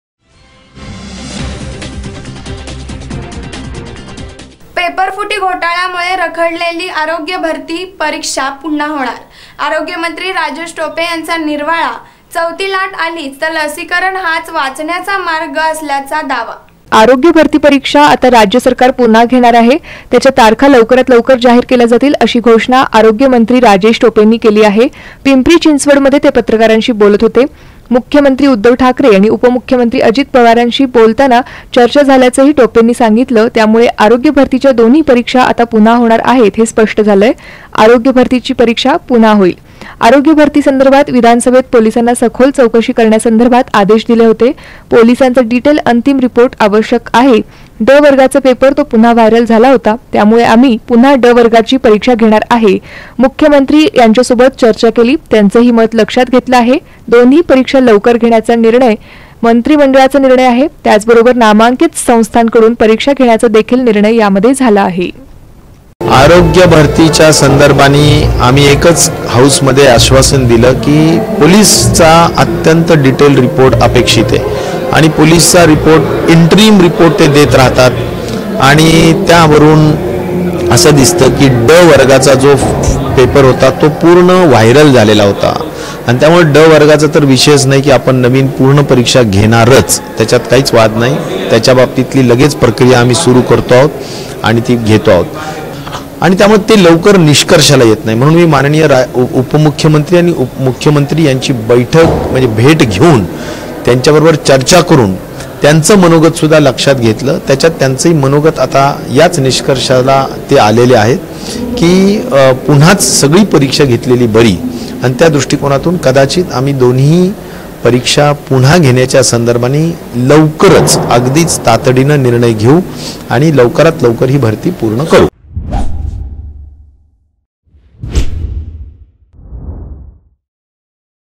PRAJESH TOPE | पेपरफुटी घोटाळ्यामुळे रखडलेली आरोग्य भरती परीक्षा पुन्हा होणार
पेपरफुटी घोटाळ्यामुळे रखडलेली आरोग्य भरती परीक्षा पुन्हा होणार, आरोग्यमंत्री राजेश टोपे यांचा निर्वाळा, चौथी लाट आलीच तर लसीकरण हाच वाचण्याचा मार्ग असल्याचा दावा